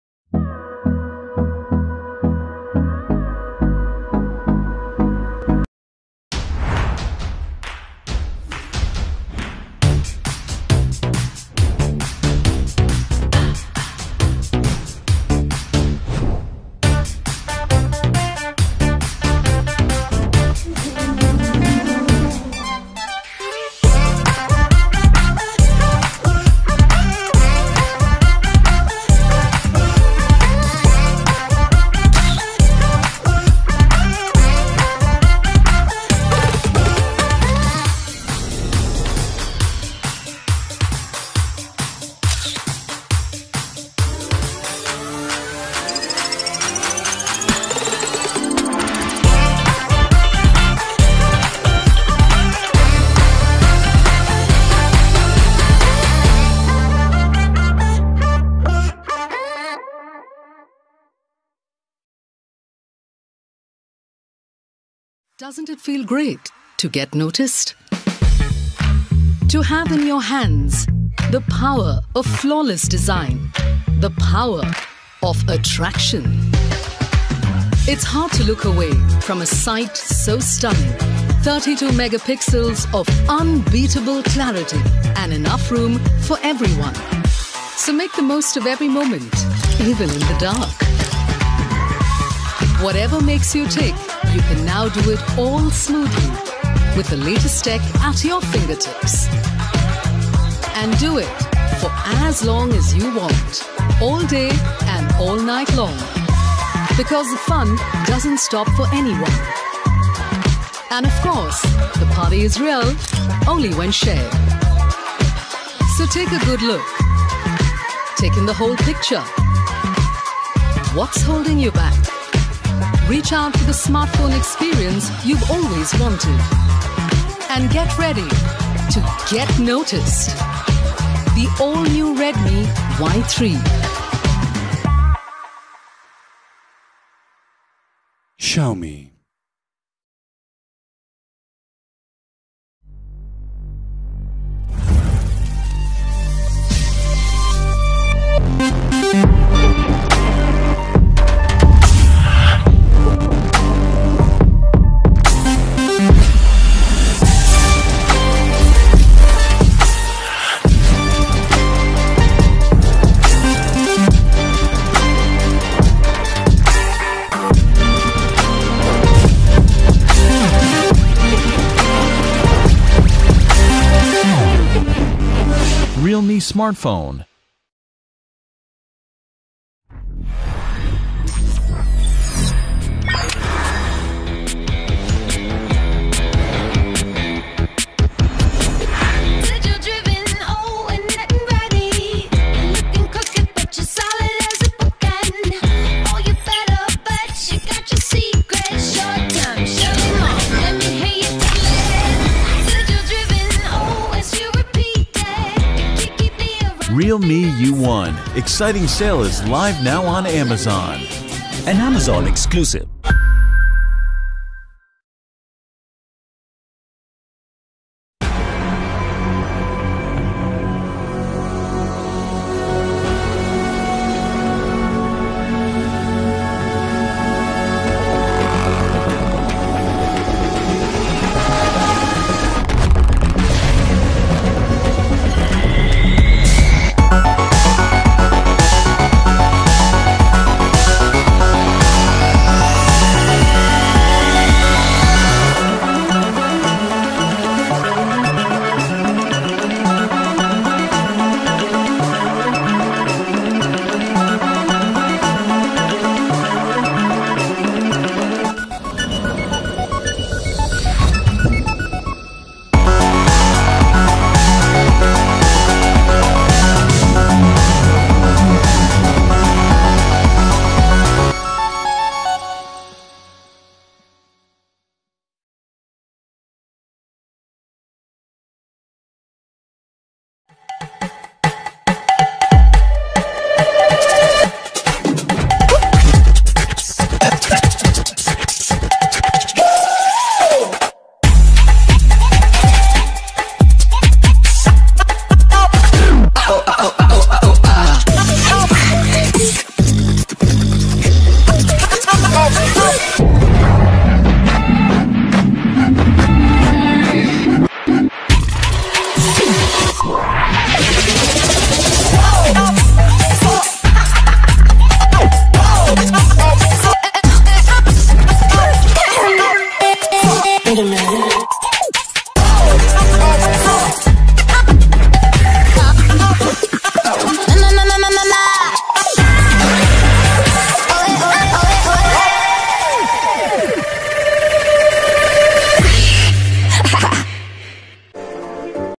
Top best phone under 12000 Rs in July August 2019
Top Best killer phone under 12000 rs. Available on flipkart, Amazon.